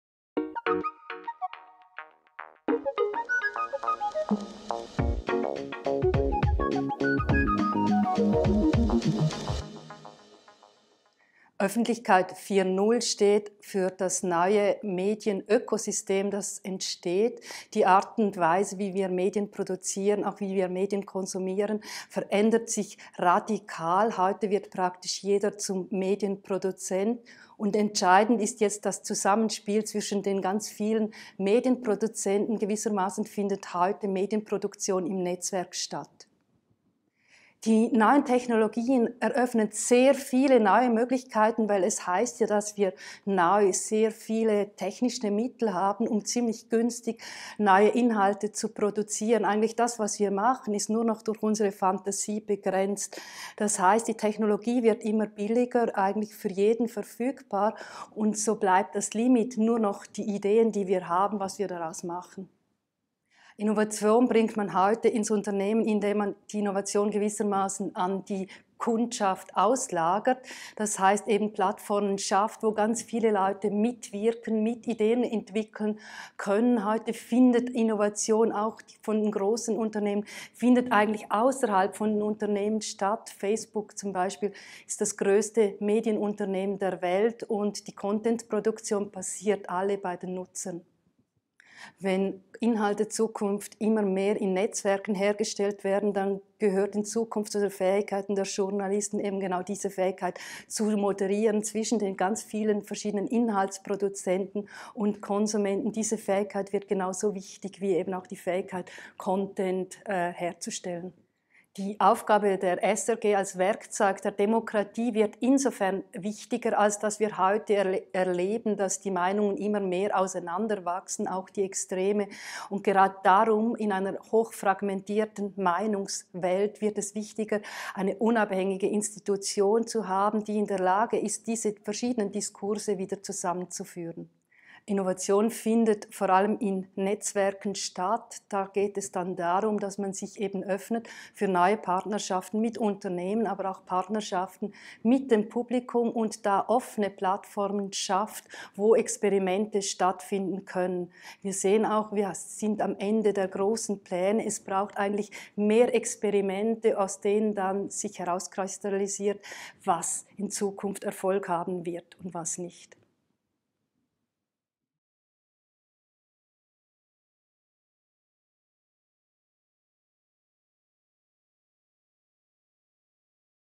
Karin Frick (Head Think Tank, GDI) zur Studie Zukunft der Medien «Öffentlichkeit 4.0»
Karin Frick, Forschungsleiterin am Gottlieb Duttweiler Institut, über die
neue GDI-Studie «Öffentlichkeit 4.0: Die Zukunft der SRG im digitalen